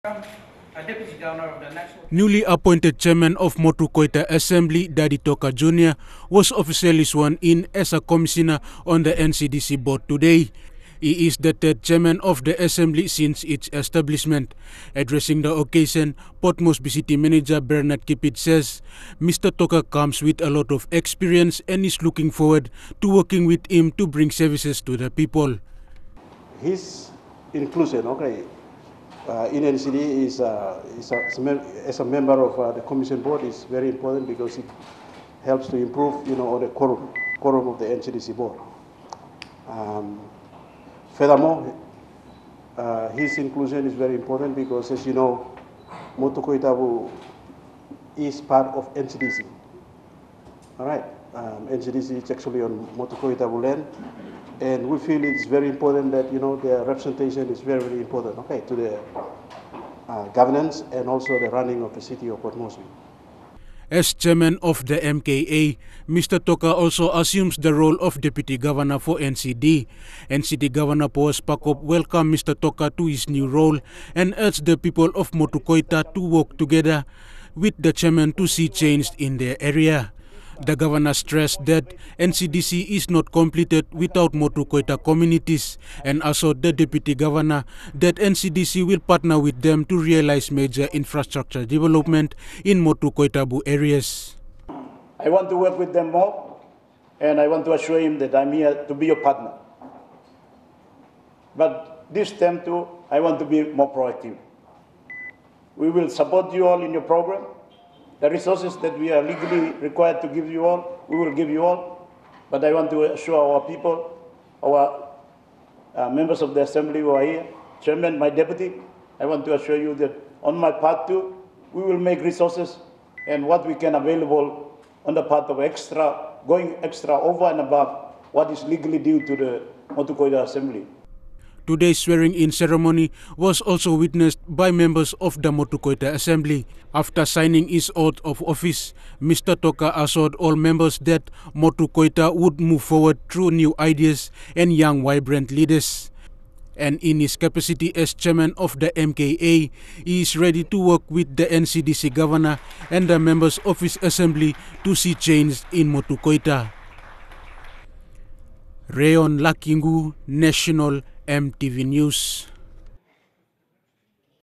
Dadi Toka Jnr signs Oath as a Commissioner of NCDC Board
The Board of National Capital District Commission today (06 November, 2018) welcomed its newest Commissioner, Dadi Toka Jnr.

Mr Toka now assumes the role as Deputy Governor following his recent election as Chairman of the Motu Koita Assembly.

In welcoming Mr Toka to his NCDC Board Role, Port Moresby City Manager Bernard Kipit promised to work together with Mr Toka for a better Motu Koita.

for more news.